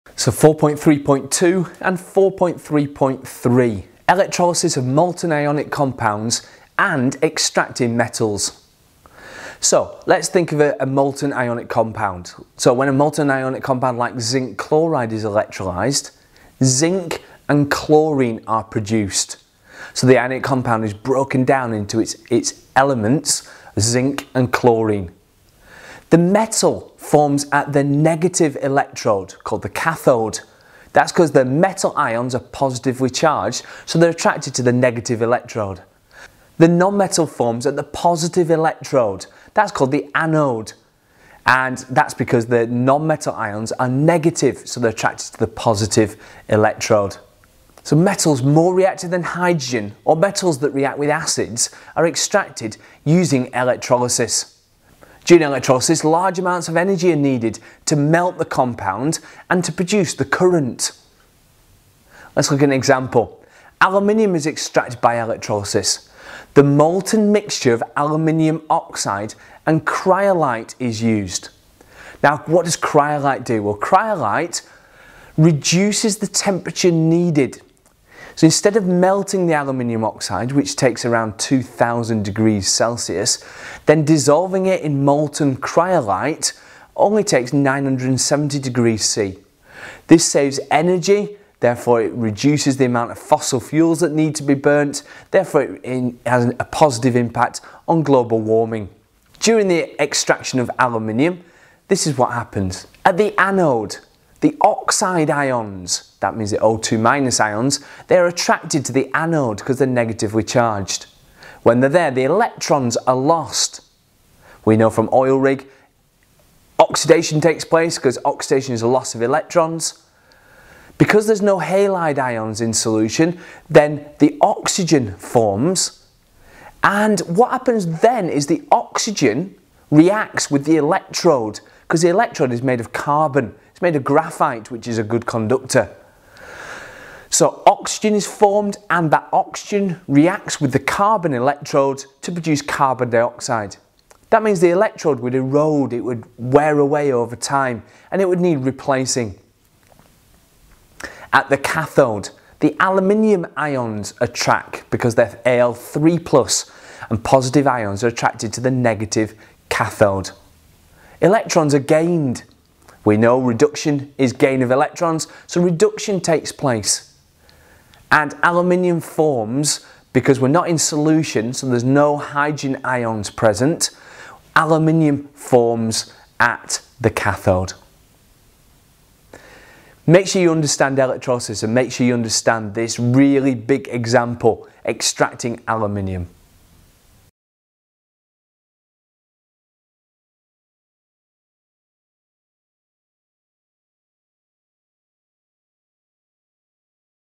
Understand C.4.3.2 and C 4.3.3 Using electrolysis to extract metals (Aluminium)
AQA Specification Notes:

C 4.3.2 Electrolysis of molten ionic compounds

When a simple ionic compound (eg lead bromide) is electrolysed in the molten state using inert electrodes, the metal (lead) is produced at the cathode and the non-metal (bromine) is produced at the anode.

Students should be able to predict the products of the electrolysis of
binary ionic compounds in the molten state.

C 4.3.3 Using electrolysis to extract metals

Metals can be extracted from molten compounds using electrolysis.
Electrolysis is used if the metal is too reactive to be extracted by reduction with carbon or if the metal reacts with carbon. Large amounts of energy are used in the extraction process to melt the compounds and to produce the electrical current.
Aluminium is manufactured by the electrolysis of a molten mixture of aluminium oxide and cryolite using carbon as the positive electrode (anode).

Students should be able to:
• explain why a mixture is used as the electrolyte
• explain why the positive electrode must be continually replaced.